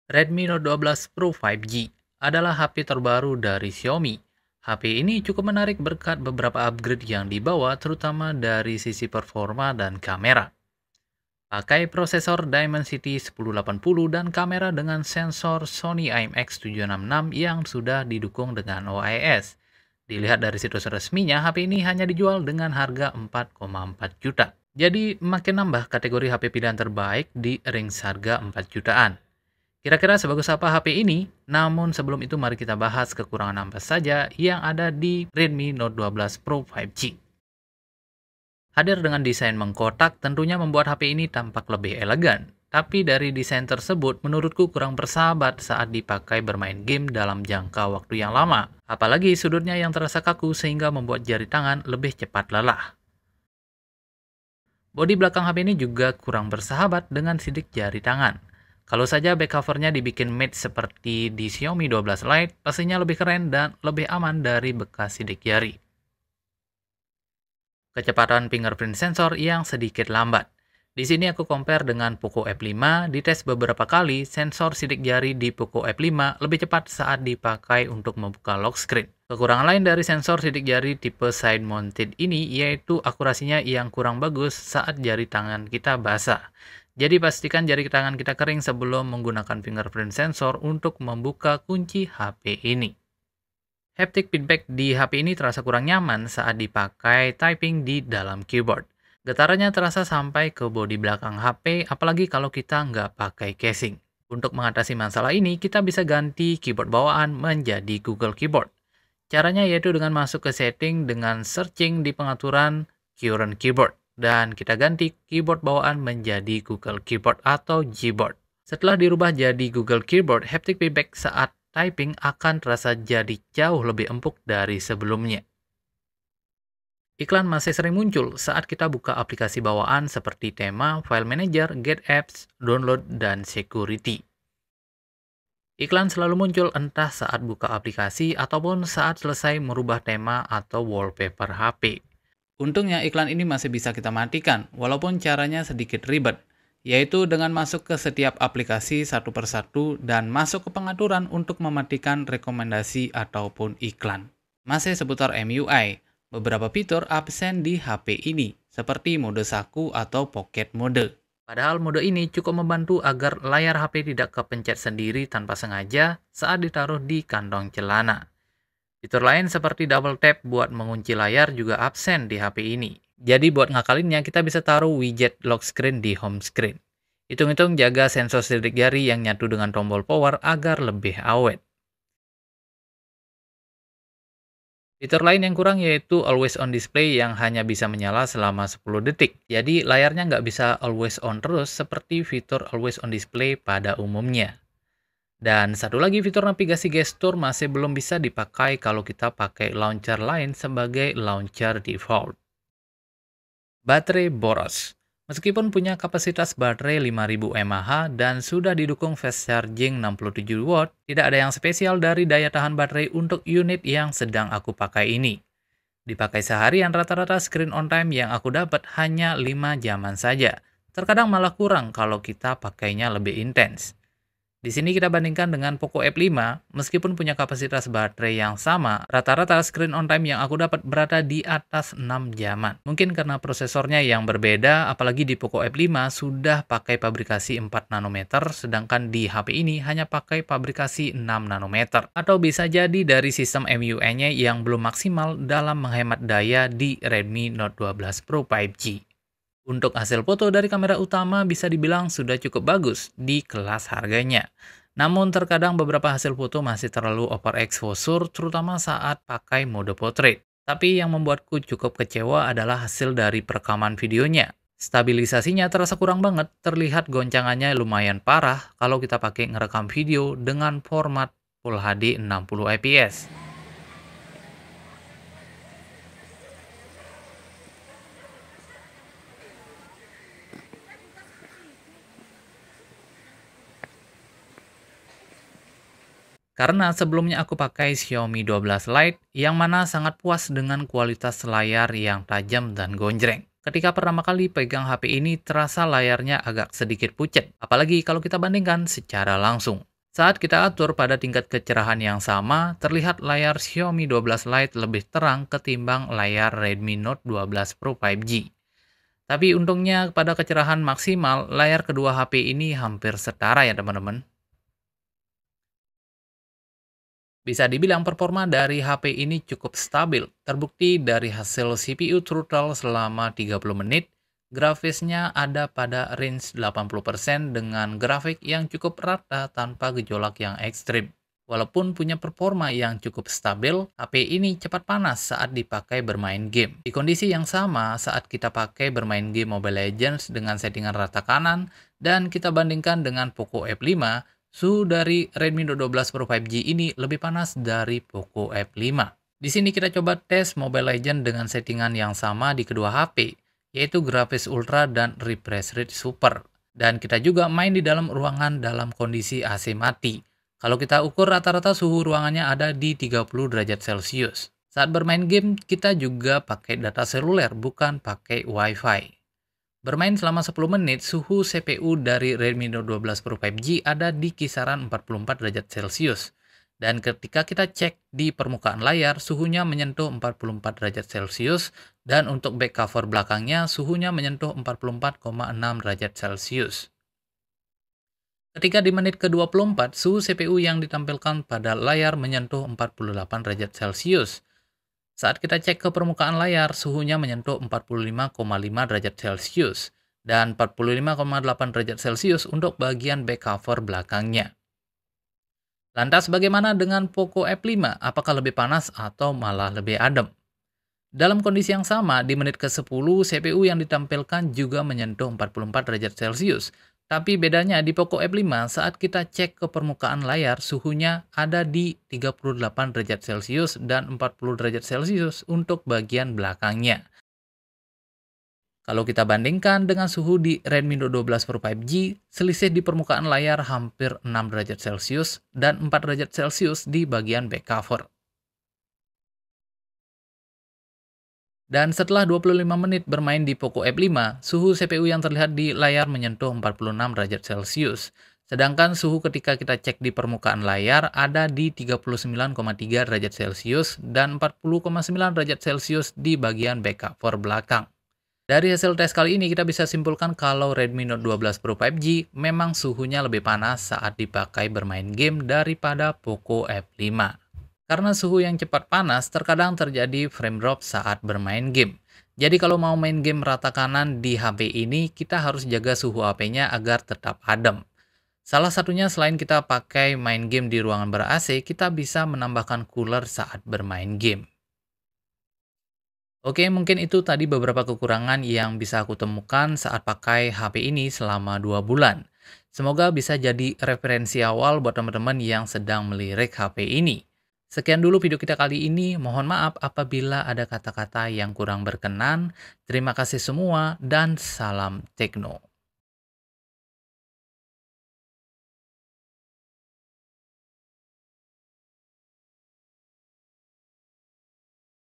Kekurangan Redmi Note 12 Pro 5G! Masih mau beli?
Hello semua balik lagi di Lapak Tekno, pada video kali ini kita akan bahas sedikit tentang kekurangan dari Redmi Note 12 Pro 5G. Pengalaman pakai redmi note 12 pro 5g selama 2 bulan disini aku menemukan beberapa kekurangan yang mungkin bisa jadi referensi buat temen temen yang mau beli HP ini. Jadi tonton videonya sampai habis ya,. Untuk tes baterai kenapa ada yang scren on timenya sedikit cuma 40 menit atau dibawah 1 jam, itu karena saat itu hpnya gak full dipakai seharian.

Kekurangan redmi note 12 pro 5G, masalah panas di redmi note 12 pro 5G, kamera redmi note 12 pro 5G, daya tahan baterai redmi note 12 pro 5g